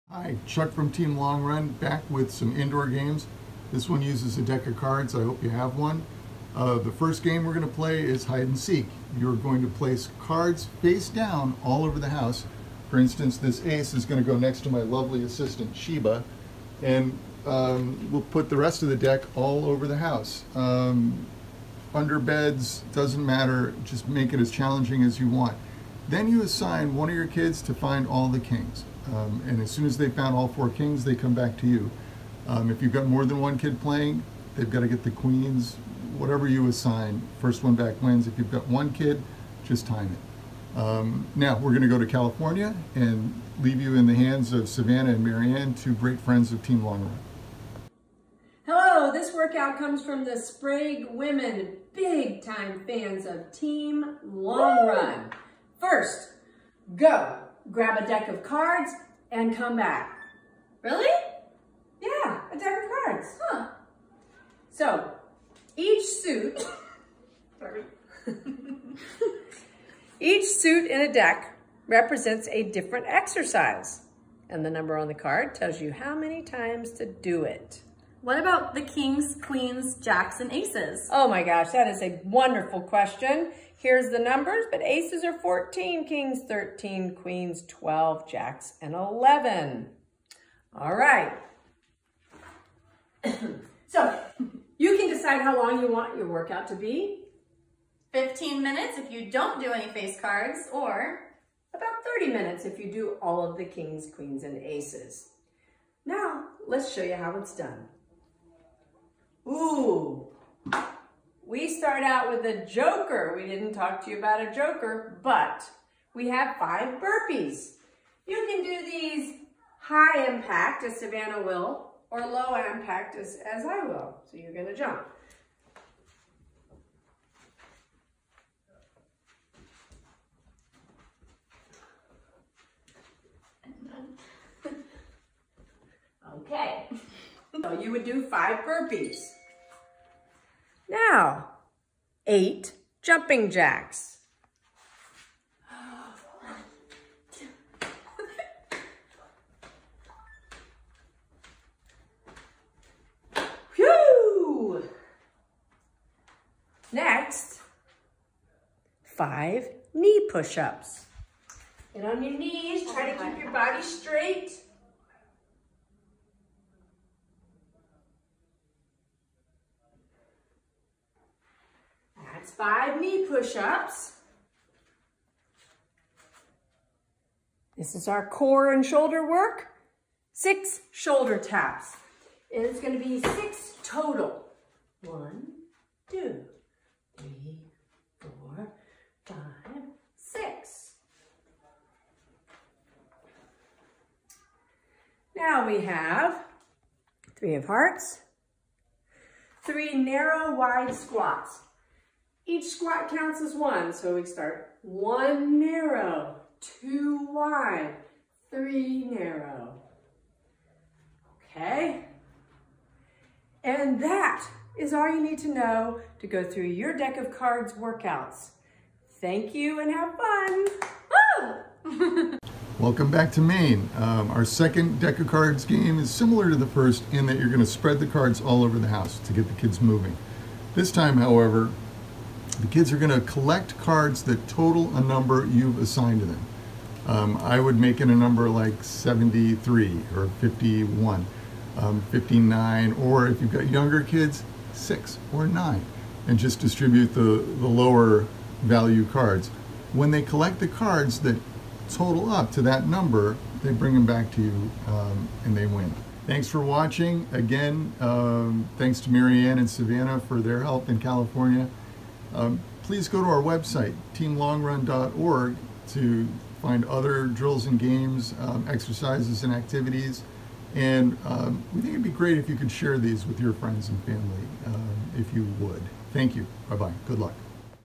TLR Indoor Recess Activities | Card Games!
Here are a few indoor "recess" activities that you can do with your kids. They're very active and don't require a lot of space. Thanks to TLR friends MA and Savvy for demonstrating their "deck of cards" game from California. Again, have fun and hang in there!

The first game we're going to play is hide and seek. You're going to place the cards face down all over the house. Under beds, on couches, it doesn't matter, you can make it as challenging as you want. Then you assign one of your kids to find all of the kings (or a different royal/number), and as soon as they've found all four they come back to you. If you've got more than one kid playing, they have to get whatever you assign, and the first one back wins. If you just have one kid - just time them!

Now we're going to head to California for our next game!

First, go grab a deck of cards, and come back!

So - each suit in a deck represents a different exercise, and the number on the card tells you how many times to do it.

Hearts = Narrow / Wide Squats
Clubs = Jumping Jacks
Diamonds = Shoulder Taps
Spades = Knees Pushups
Jokers = 5 Burpees

What about the Aces, Kings, Queens, Jacks? Good question! The Aces are 14, the Kings 13, Queens 12, and Jacks 11.

Alright, so, you can decide how long you want your workout to be! 15 minutes if you don't do any face cards, or about 30 minutes if you do all of the Kings, Queens, Jacks, and Aces.

That's all you need to know to go through your deck of cards workout!

Alright, now we're back to Maine! Our final deck of card's game is similar to the first in that you're going to spread the cards all over the house to get the kids moving. This time, however, the kids are going to collect cards that total the number you've assigned to them. I would make it a number like 73. If you've got younger kids, something like 9, and just distribute the lower value cards.

When they collect the cards that total up to that number they bring them back to you - and they win!

Please go to teamlongrun.org for other drills/games/activities for kids at home. Goodbye and good luck!